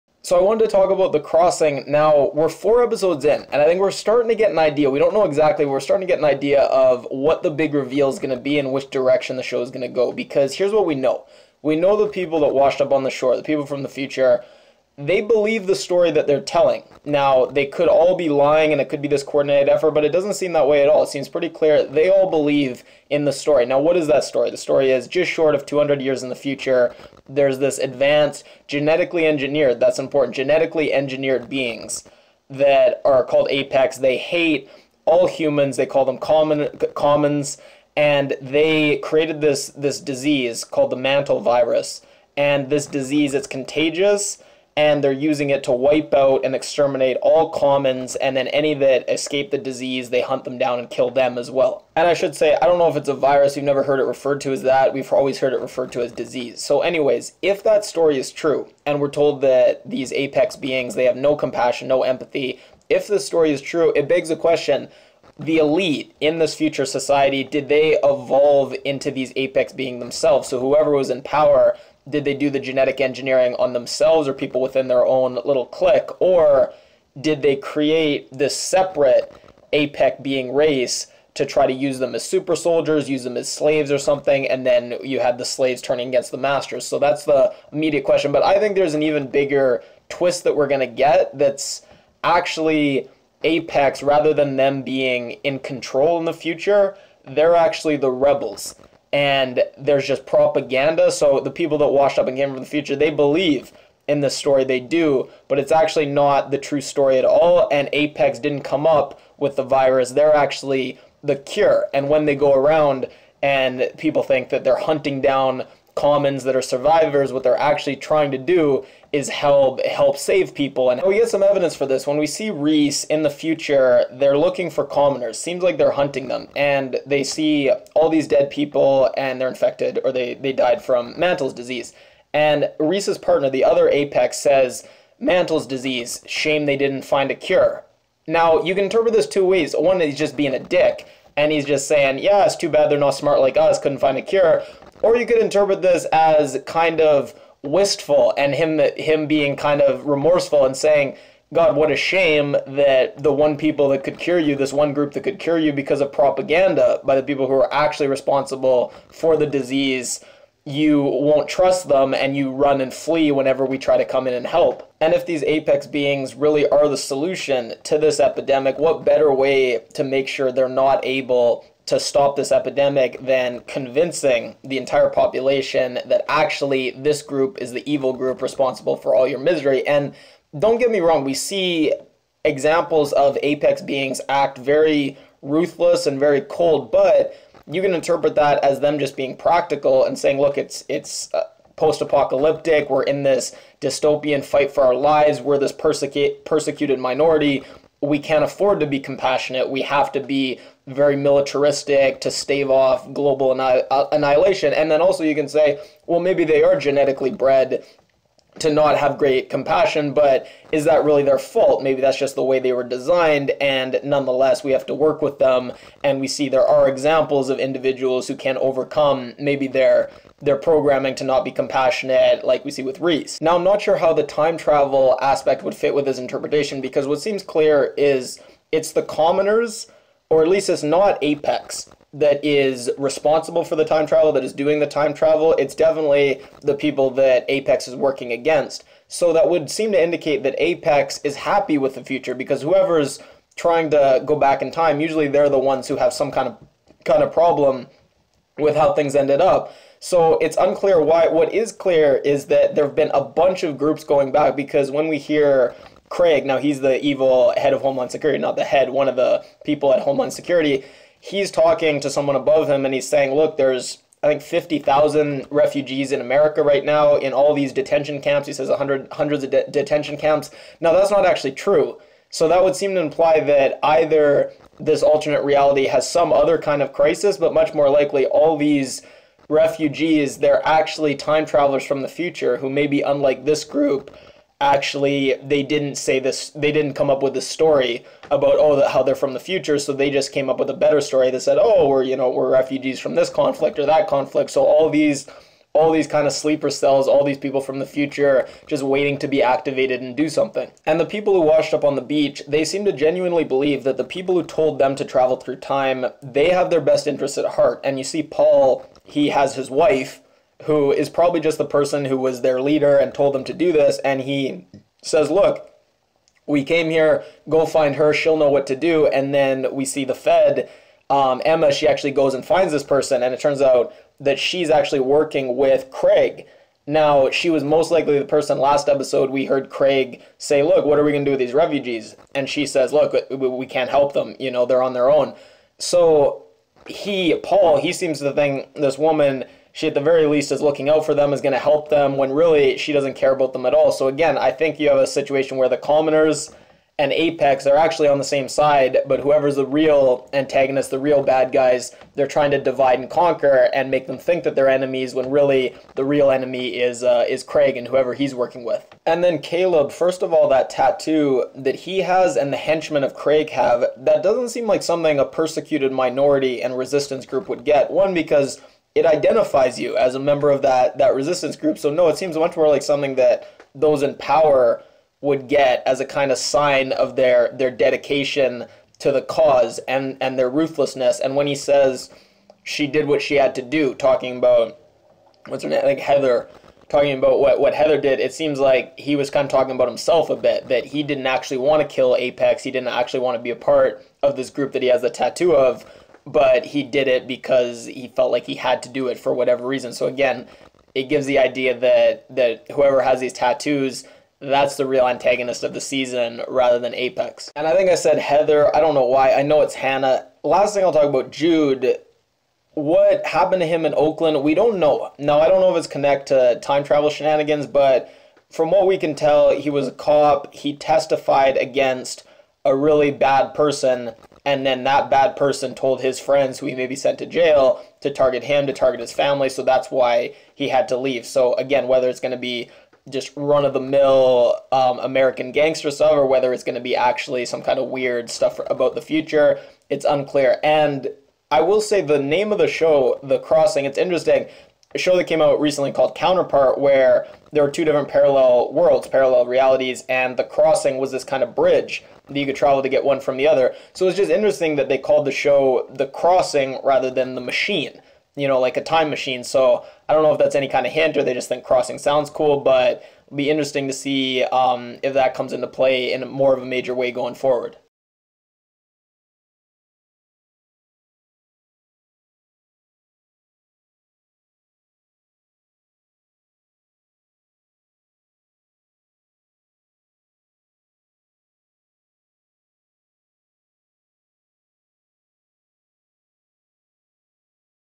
The Crossing: Apex Theory / Episode 4 Ending Recap "The Face of Oblivion"
We still don't know what "the Long Peace" is, or what the Alpha-Zulu tattoo means (Caleb has one), or what happened with the Doucette case that Jude is running from in Oakland.
 The year the refugees came from is ~2195, 177 years in the future.
Earth in the future is Apex (genetically engineered, immune to Mantle’s disease) vs COMMONS. Craig Lindauer is Homeland guy, SOPHIE is trying to help infected LEAH (all the names are biblical).
 ROY has a crush on HANNAH (who is from Metolius) and has Marshall's photo in her locket.OLIVER is JUDE'S SON. Emma Wren gets shot. THOMAS is informant,

This is my review / recap for episode 4 of The Crossing as well as an ending explanation called "The Face of Oblivion"